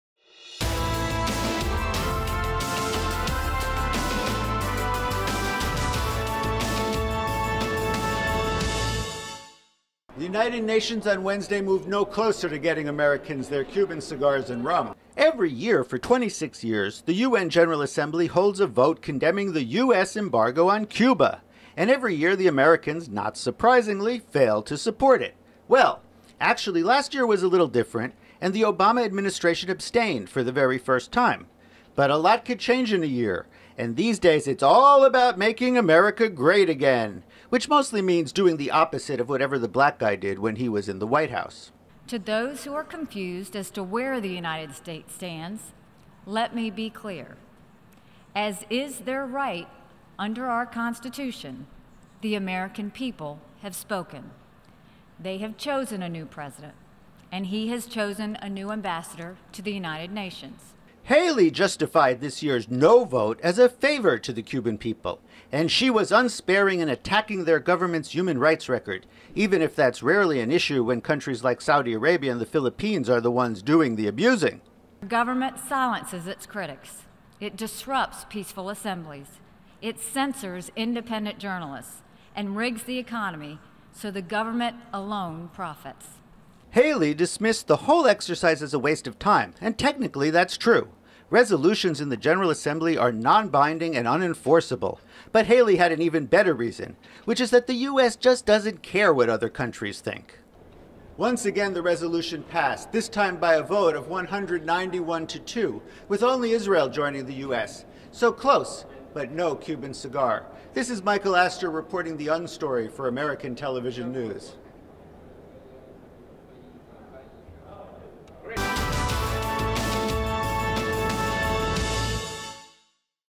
the UN story Americans No Closer to Cuban Cigars and Rum
The United Nations on Wednesday moved no closer to getting Americans their Cuban cigars and rum.
Every year for 26 years, the UN General Assembly holds a vote condemning the US embargo on Cuba and every year the Americans, not surprisingly, fail to support it.
Well, actually, last year was a little different and the Obama administration abstained for the very first time.
But a lot can change in a year and these days it’s all about making America great again, which mostly means doing the opposite of whatever the black guy did when he was in the White House.